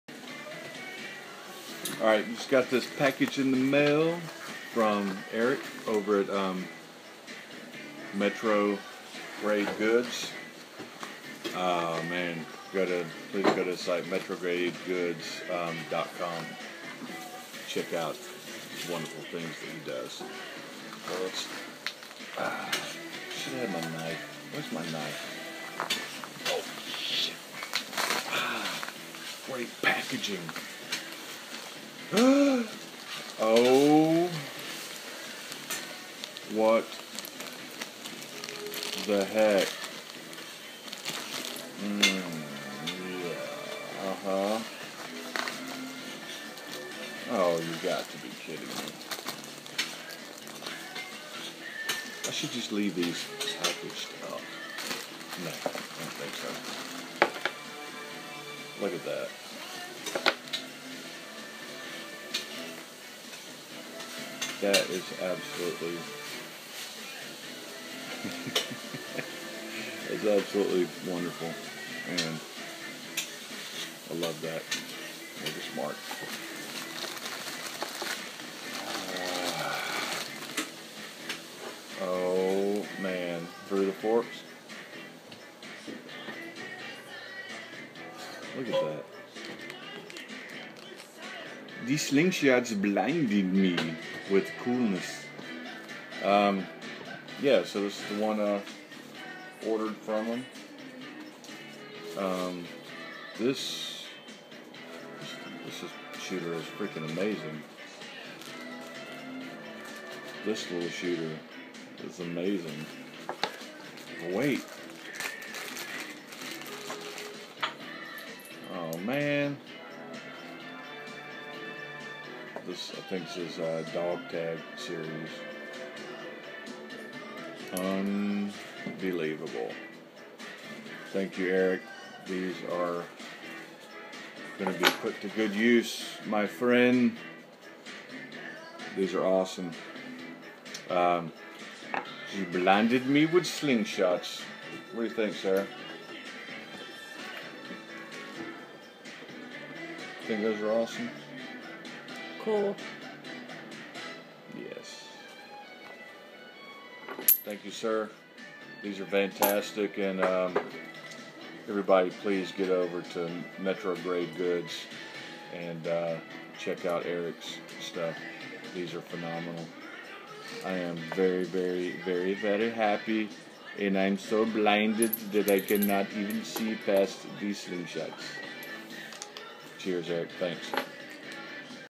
Metro Grade Goods Awesomeness
Some wonderful Goodness from Eric Au over at Metrogradegoods.com. go check him out. Fantastically Brilliant
TTF Capuchin
A Jester
Dog Tabs
Thanks for these Awesome Slings